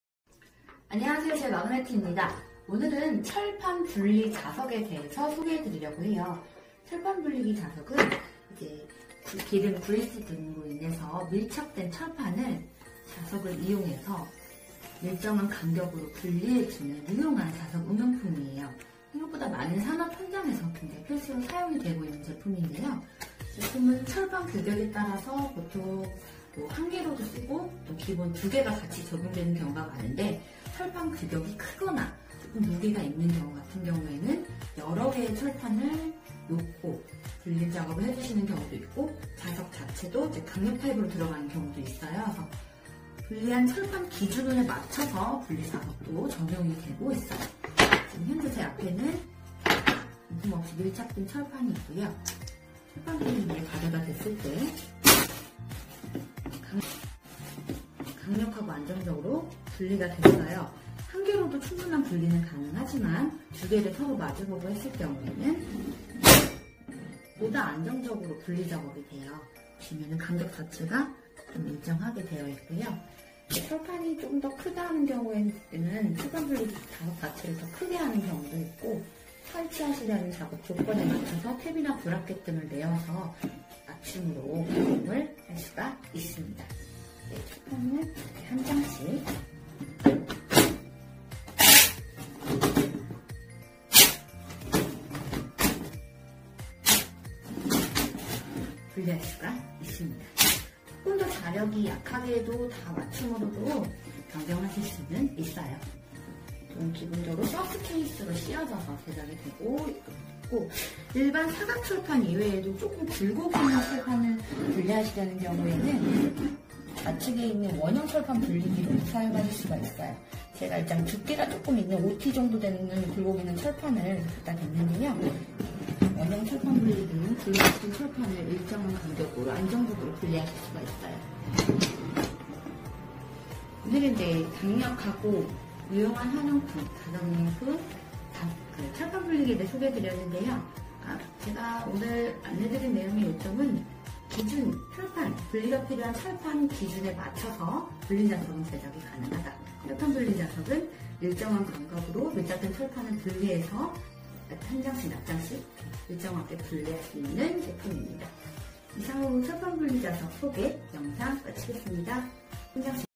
왜이리 안떨어지는것이냐
▶철판분리자석
현장에서 작업을 하다보면 기름 등으로 인해 철판이 밀착되어
안떨어지는 경우를 볼 수 있는데요! 손으로 떼려다보면 분리하다 다치거나
분리 하는 과정으로 인해 작업시간이 지연되는 경우가 있습니다.
이럴때에는 마그네틱을 특수배열하여 밀착 된 자석을 분리해주는
철판분리기(철판 분리자석)를 사용하시면 보다 안정적이고 손쉽게 철판을
분리해줍니다. :)

▶사용제품
0:00~0:47 철판분리자석 특징
0:48~1:50 분리테스트
1:51~2:43 제작품 소개